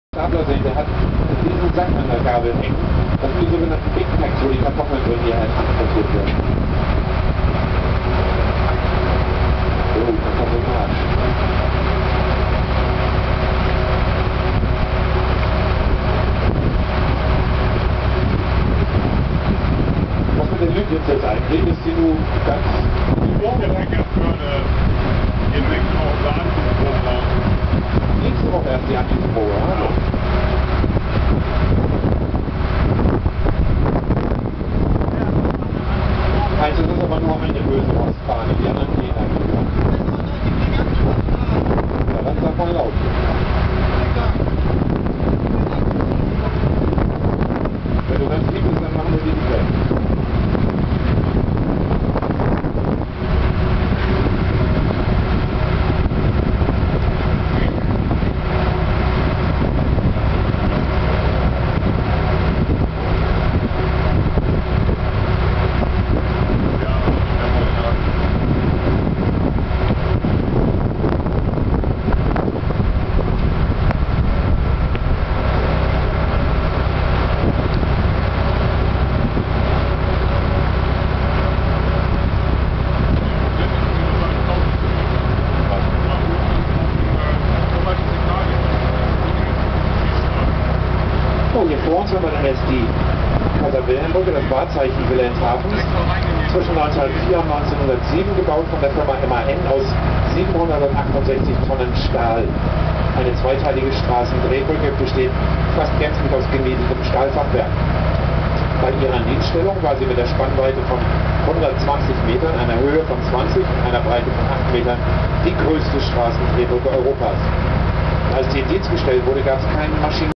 D186 Zerstörer Mölders im Marinemuseum Wilhelmshaven
Alle Jahre wieder eine PönEx auf D186 Zerstörer Mölders im Marine-Museum in Wilhelmshaven. Dieser Film ist aus 2006.
Die Qualität ist leider miserabel, da ich nur eine Foto-Digi-Cam dabei hatte. Sorry.